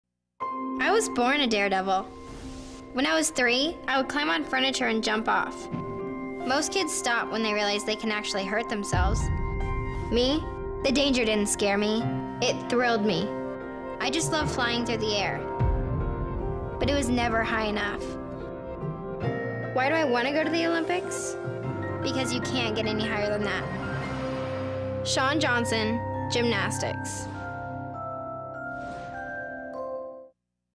Shawn Johnson NBC Promo 1
Promo run on NBC in the run up to the Olympics.